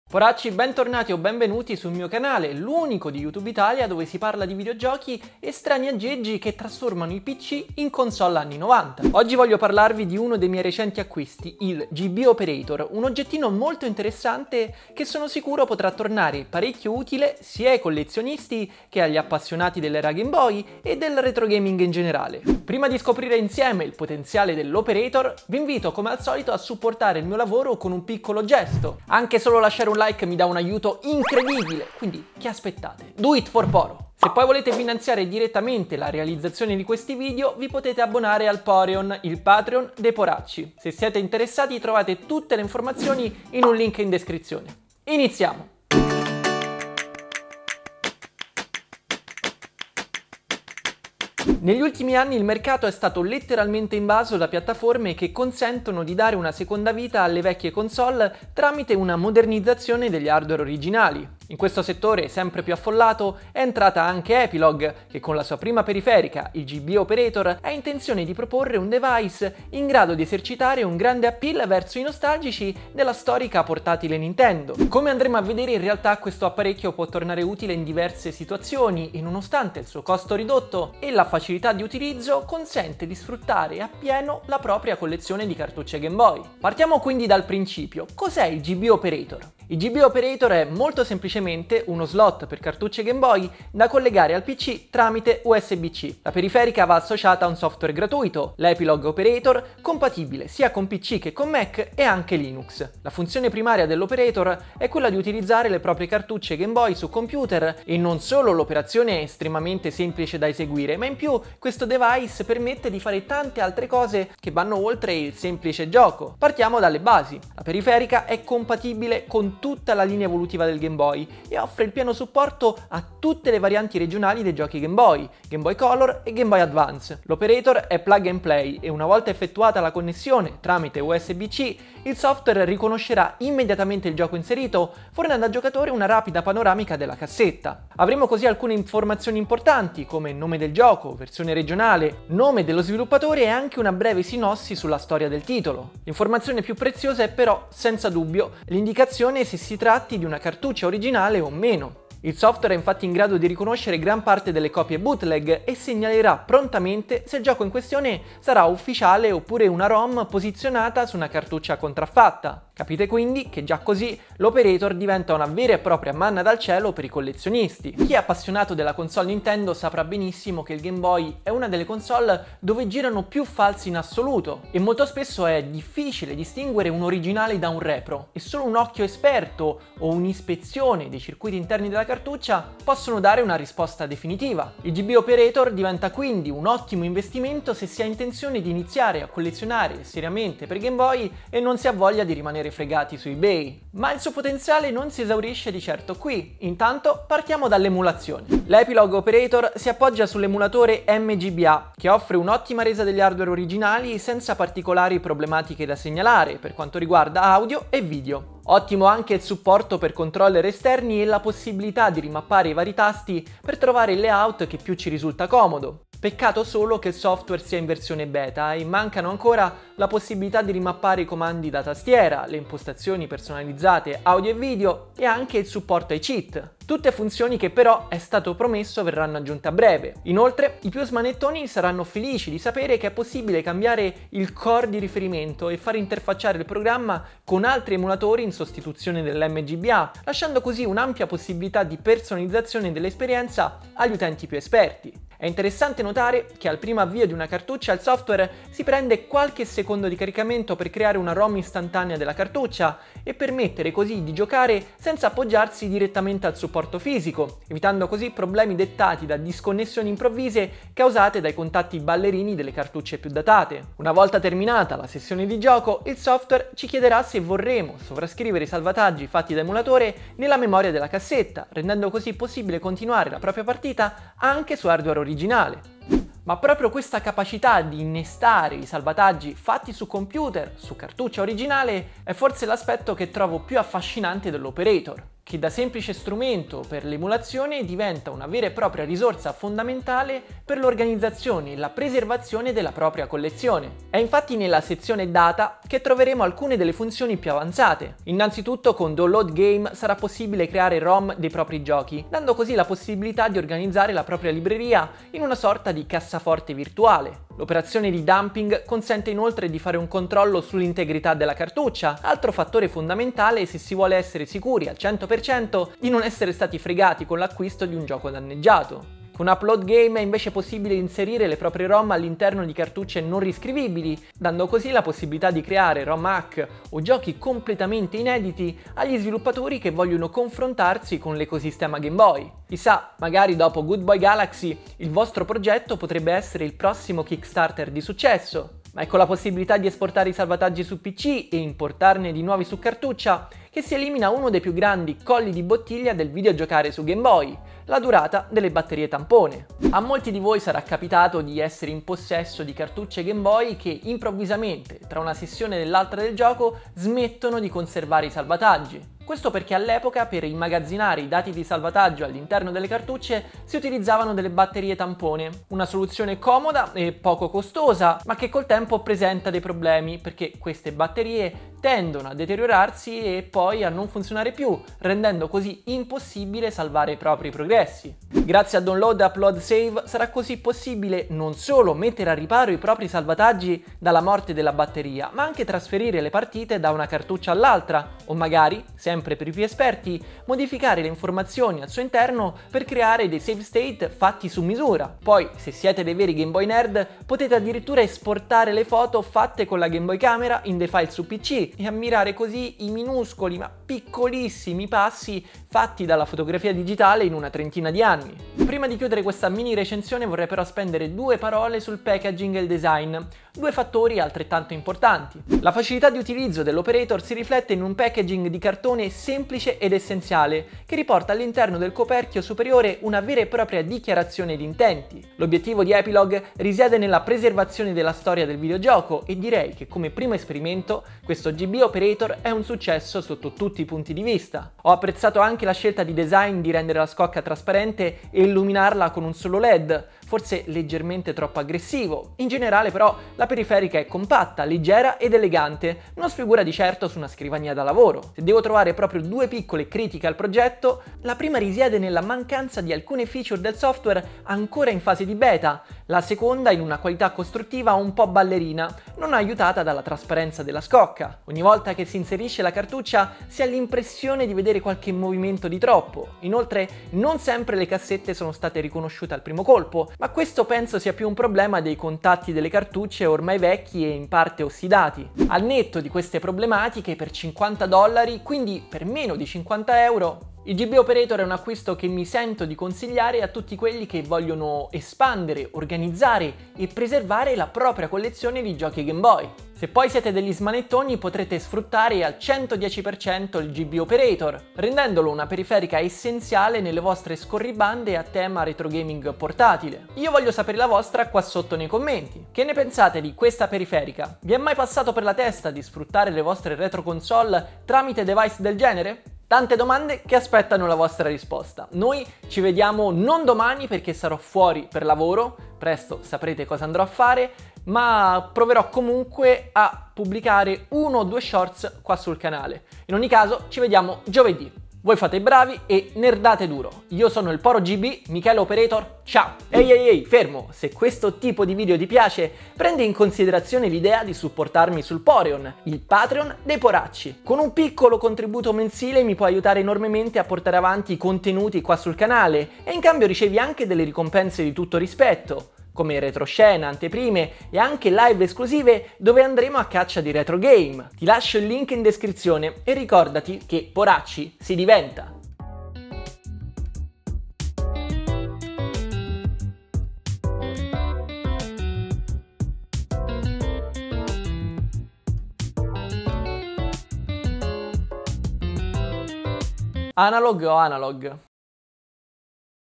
GIOCA LEGALMENTE AL GAME BOY SU PC E SALVA LA TUA COLLEZIONE POKÉMON [GB OPERATOR]
Poracci bentornati sul mio canale! Oggi vi parlerò del GB Operator una periferica compatibile con PC, Mac e Linux che permette di trasformare il computer in un Game Boy. Grazie a questa periferica potrete emulare legalmente le vostre cartucce su PC e inoltre potrete mettere in un luogo sicuro i salvataggi dei vostri giochi Pokémon senza timore che la batteria tampone vi abbandoni prima o poi. Cosa ne pensate?

00:00 Intro
00:52 Come sfruttare al 100% le retroconsole
01:15 Cos'è il GB Operator?
02:47 Come distinguere immediatamente una repro - fake da un gioco originale
03:18 Emulazione Game Boy LEGALE con mGBA
04:36 Dump e salvataggio file state: addio batterie tampone
05:49 Perché le mie cartucce Game Boy non salvano?
06:48 Packaging e Design
07:28 Difetti del GB Operator
07:55 GB Operator: vale la pena?
08:20 Outro e Patreon